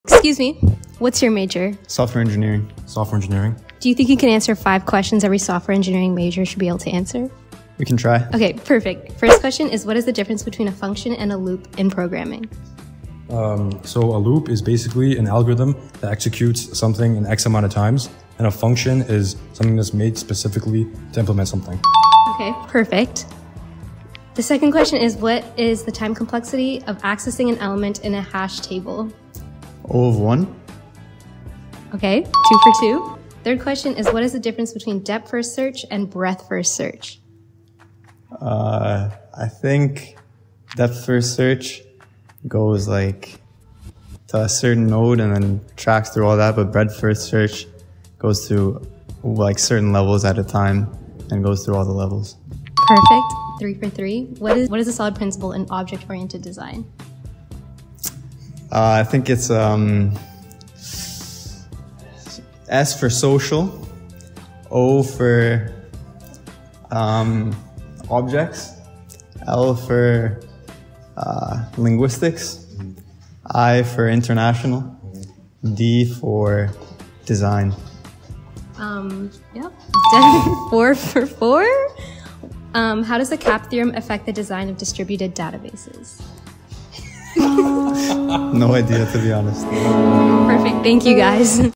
Software Engineering Major Trivia | Campus Interviews
Can You Answer These 5 Questions Every SE Student Should Know? We put these Software Engineering majors to the test with rapid-fire coding trivia. From debugging basics to system-design smarts, see how well they really know their major! 📸

Interviewed By: Tasiya Nadavallil
Edited By: Williams Thottungal